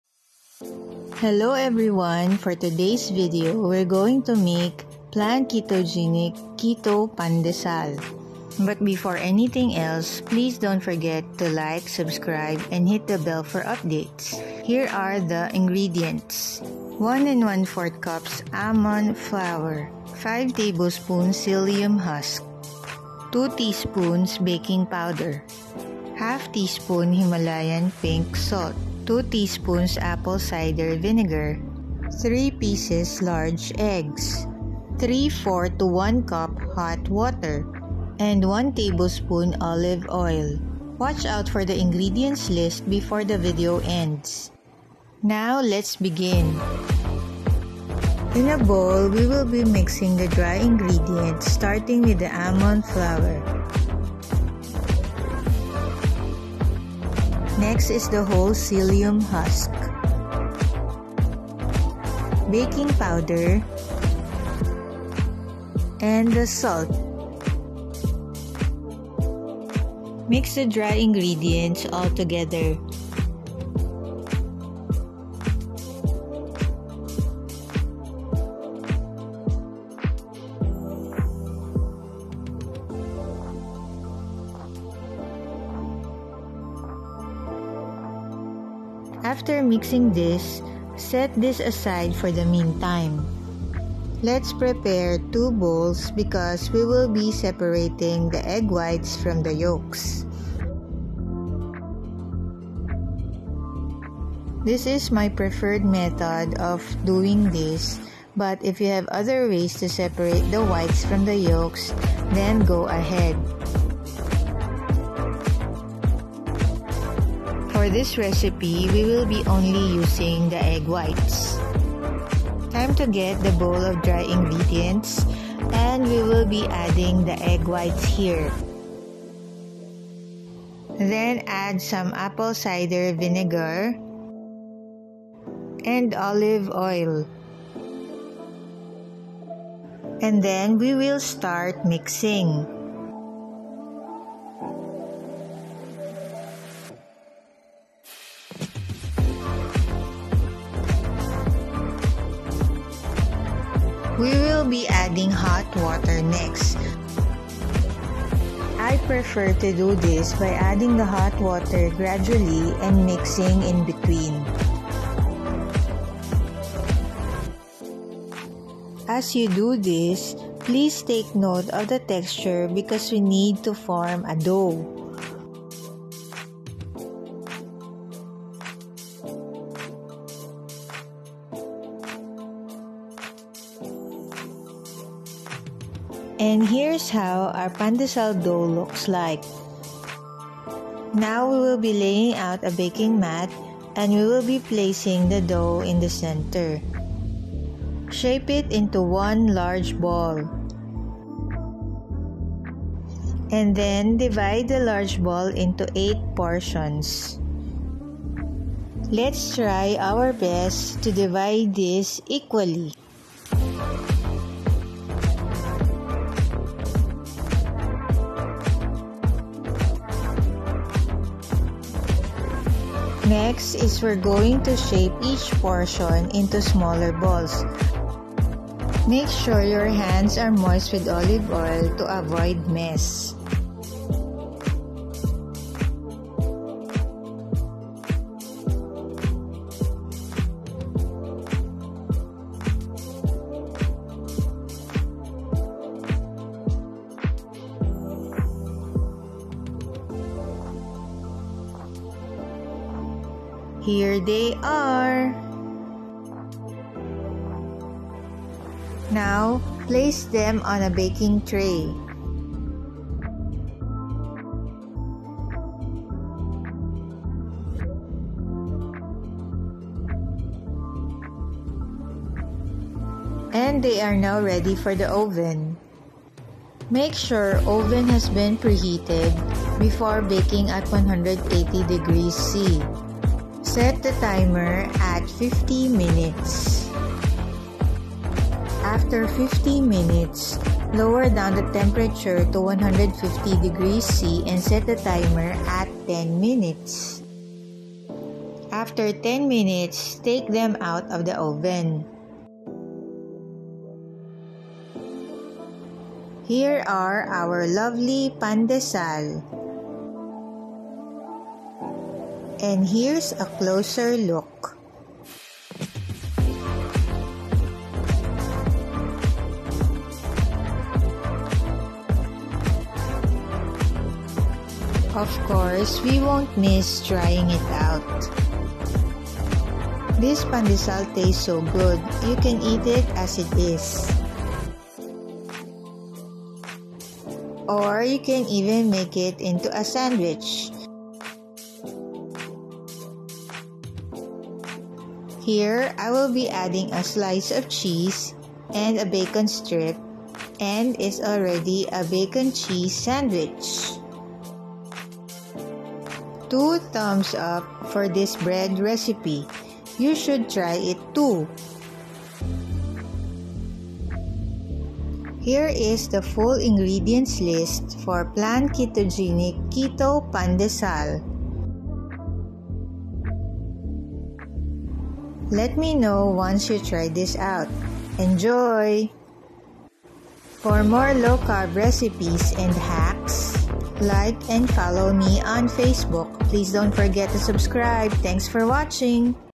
Easy Pandesal Recipe (Low Carb/LC/Keto) PlanKetogenic | Misis B's Cube Philippines 🇵🇭
I tried countless times making this well-loved Filipino bun but would always end up disappointed. That is why I am so happy to have found and tried this awesome recipe from PlanKetogenic.

00:01 Hello, everyone! For today's video we're going to make
PlanKetogenic Keto Pandesal.
00:09 But before anything else, please don't forget to like, subscribe and hit the bell for updates.
00:15 Here are the ingredients.
00:18 One and one-fourth cups Almond Flour
00:20 Five tablespoons Psyllium Husk
00:23 Two teaspoons Baking Powder
00:26 Half teaspoon Himalayan Pink Salt
00:29 Two teaspoons Apple Cider Vinegar
00:32 Three pieces large Eggs
00:35 Three-fourth to one cup HOT Water
00:38 and one tablespoon Olive Oil
00:41 Watch out for the Ingredients List before the video ends.
00:45 Now let's begin
00:48 In a bowl we will be mixing the dry ingredients starting with the Almond Flour.
00:58 Next is the Whole Psyllium Husk
01:02 Baking Powder
01:06 And the salt
01:11 Mix the dry ingredients altogether.
01:29 After mixing this, set this aside for the meantime.
01:34 Let's prepare 2 bowls because we will be separating the egg whites from the yolks.
01:44 This is my preferred method of doing this but if you have other ways to separate the whites from the yolks, then go ahead.
01:58 For this recipe we will only be using the egg whites.
02:02 Time to get the bowl of dry ingredients and we will be adding the egg whites here.
02:11 Then add some Apple Cider Vinegar
02:16 And Olive Oil
02:20 And then we will start mixing
02:35 We will be adding hot water next.
02:39 I prefer to do this by adding the hot water gradually and mixing in between.
02:49 As you do this, please take note of the texture because we need to form a dough.
03:06 And here's how our pandesal dough looks like
03:11 Now we will be laying out a baking mat and you will be placing the dough in the center.
03:18 Shape it into one large ball.
03:23 And then divide the large balls into 8 portions.
03:29 Let's try our best to divide this equally.
03:43 Next is we're going to shape each portion into smaller balls.
03:49 Make sure your hands are moist with olive oil to avoid mess.
04:14 Here they are...
04:19 Now, place them on a baking tray.
04:33 And now they are ready for the oven.
04:37 Make sure oven has been pre-heated before baking at 180 degrees C
04:43 Set the timer at 50 minutes
04:49 After 50 minutes, lower down the temperature to 150 degrees C and set the timer at 10 minutes.
04:59 After 10 minutes, take them out of the oven
05:06 Here are our lovely pandesal.
05:12 And here's a closer look
05:25 Of course, we won't miss trying it out.
05:30 This pandesal is so good. You can eat it as it is.
05:37 Or you can even make it into a sandwich.
05:45 Here I will be a slice of cheese and a bacon strip and it's already a bacon cheese sandwich.
05:56 Two-thumbs up for this bread recipe. You should try it too.
06:05 Here is the full ingredients list for PlanKetogenic Keto Pandesal
06:15 Let me know once you try this out. Enjoy.
06:20 For more Low Carb Recipes and Hacks, Like and follow meon facebook. Please don't forget to subscribe. Thanks for watching.